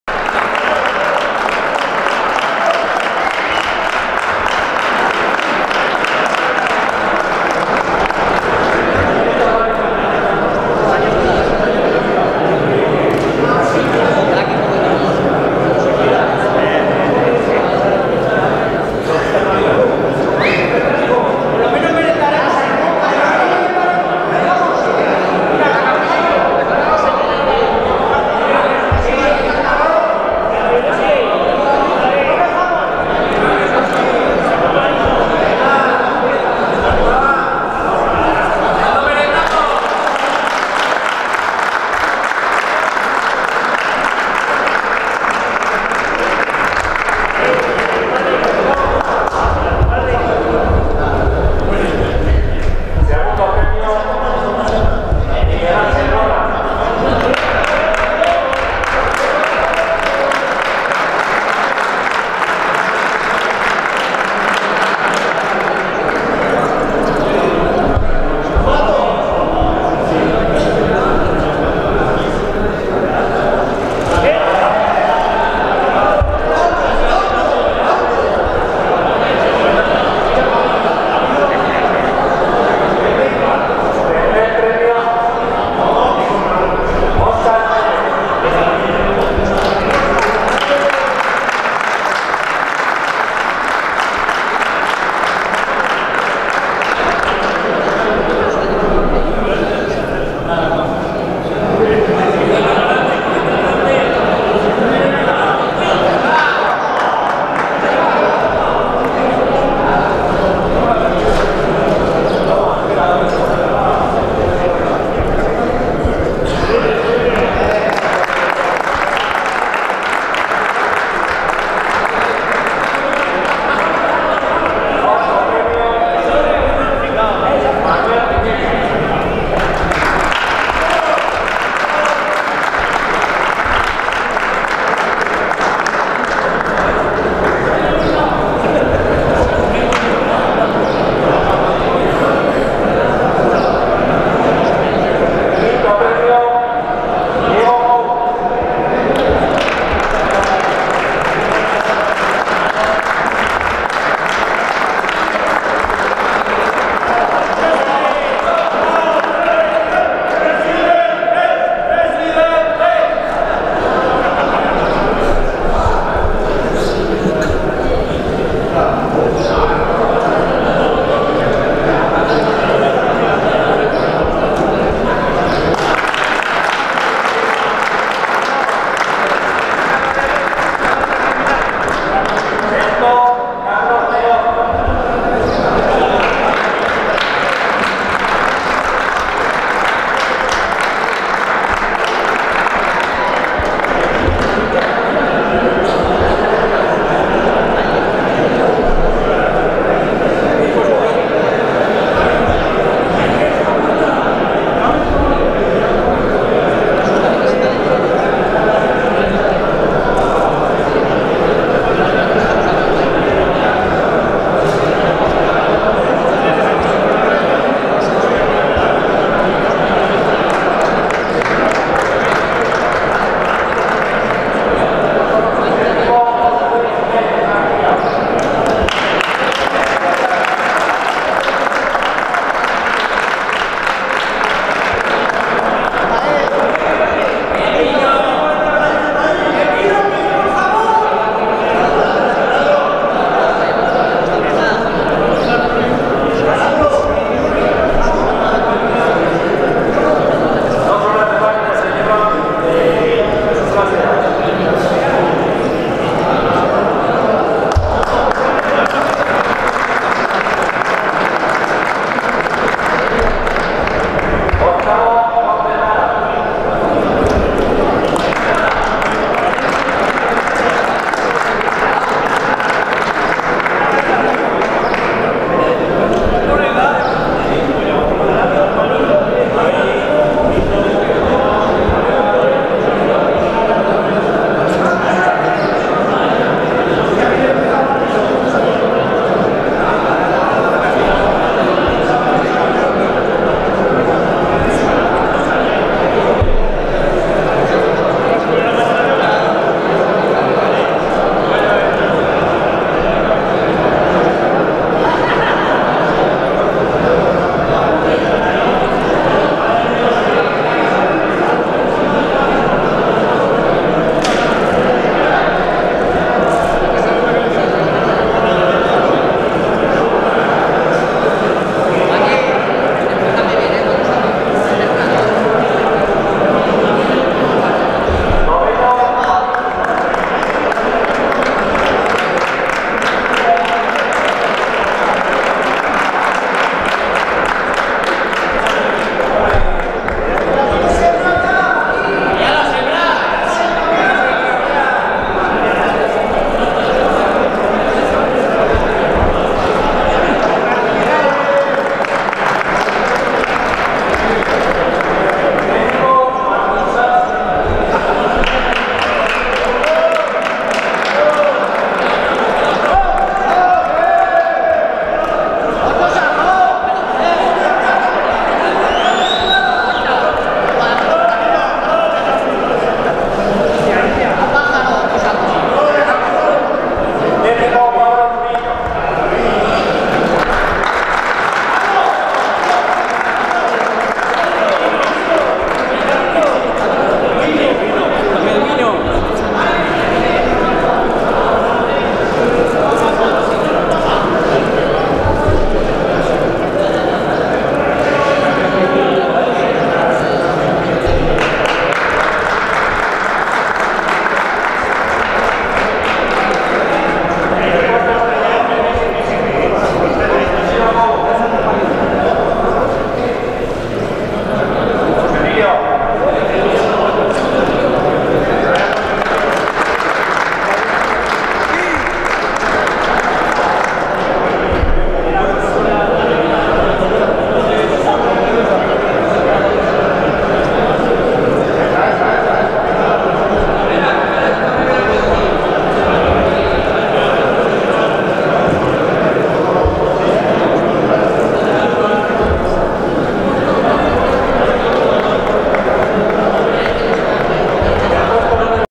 Entrega de premios 2
Cooperativa agrícola San Atilano de Tarazona, Premio de la Confederación Española de Pequeños y Medianos Empresarios (CEPYME) al ejemplo del alma de la pequeña y mediana empresa en la Comarca de Tarazona y el Moncayo, entre otros valores: por su asentamiento en el territorio, la trayectoria empresarial, la innovación y la economía social. El premio se entregó el día 12 de Diciembre del 2016 a las 18 horas en la sala de la Corona del Gobierno de Aragón.